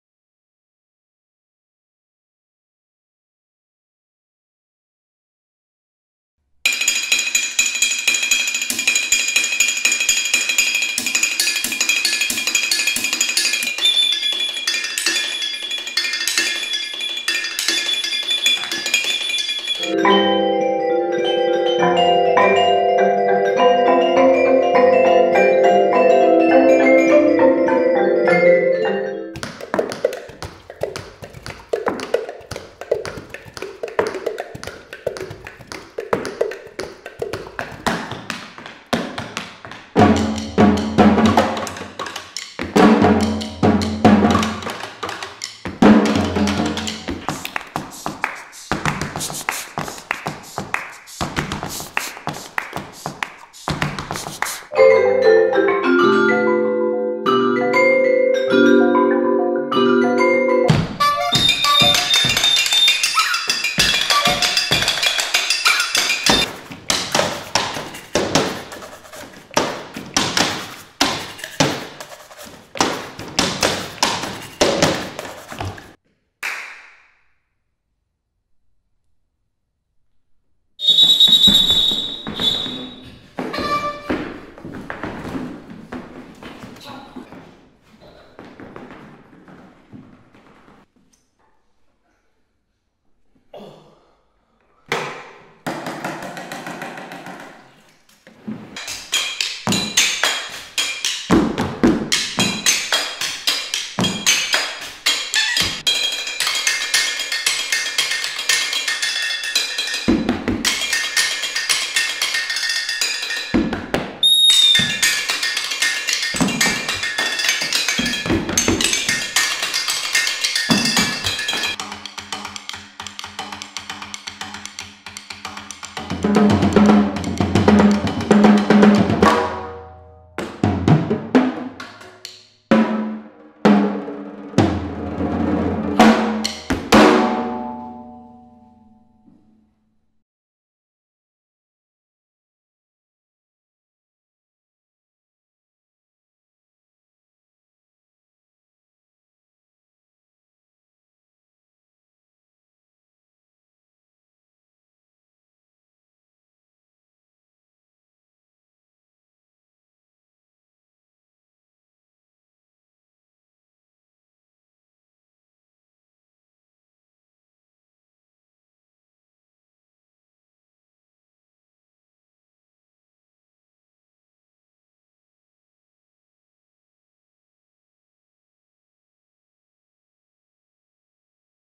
"IDEAS, NOT THEORIES" By Reynaliz Herrera. OFFICIAL TRAILER.
"Ideas, Not Theories"

OFFICIAL TRAILER from the full show.

Creator/Director/Composer/Script/Main performer: Reynaliz Herrera.
Director of Photography: Armando J. Perez- Carreno

Performers: Reynaliz Herrera, Lissy Ortega, Daniel Brizuela, Edgar Yanez.

"IDEAS, NOT THEORIES, is a Percussion Performance Concert created by Reynaliz Herrera. Combining percussion music and theatrical elements, the performance feautures original music (written by Reynaliz) which explores the use of unconventional instruments such as: bikes, tap and body percussion, water, etc, and traditional percussion instruments such as marimbas and drums. All of the musical works are linked together through a storyline and original characters, making the complete performance a unique, quirky and creative artistic experience."